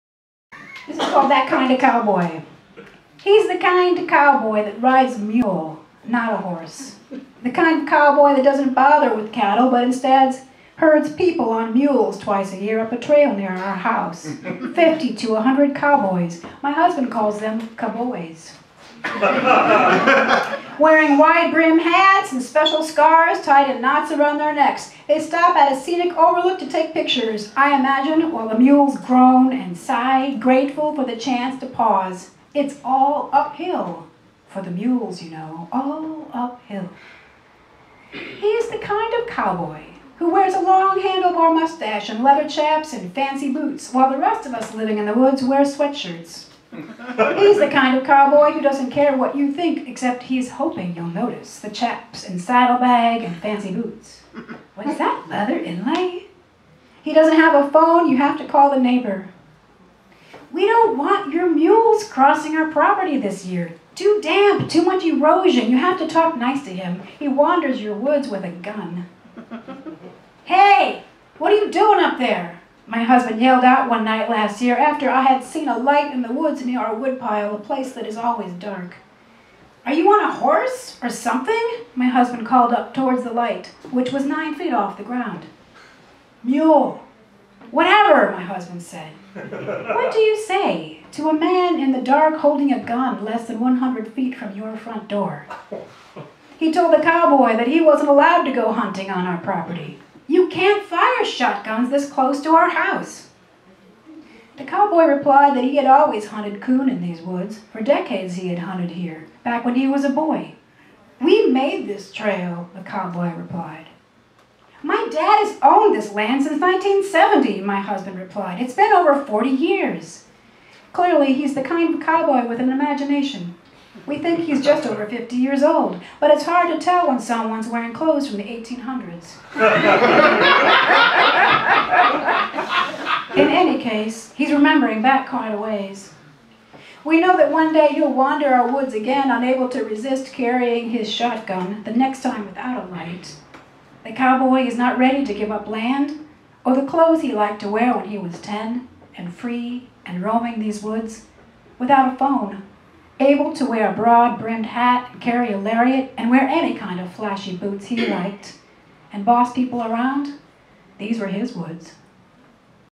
"That Kind of Cowboy" by June Melby
June Melby is an author and a performer, a former standup comedian, and slam poet. Her memoir My Family and Other Hazards (Holt 2014) was on the New York Times Bestseller list and won the Midwest Connections Award. It tells the story of the retro 1950s miniature golf course where her family lived and waited on tourists who roamed their backyard.

Her work has appeared in McSweeney's Internet Tendency, Utne Reader, LAWeekly, and journals including Versal and Forklift Ohio.She loves to travel and has performed in cities all over the world, from Los Angeles, to London, Munich, Hamburg, and Amsterdam. She teaches creative writing at a number of residencies and conferences. She received an MFA in creative writing from the University of Iowa, and then moved to a log cabin in the woods where she lives with her husband and a 20 pound cat named Ferdinand Magellan.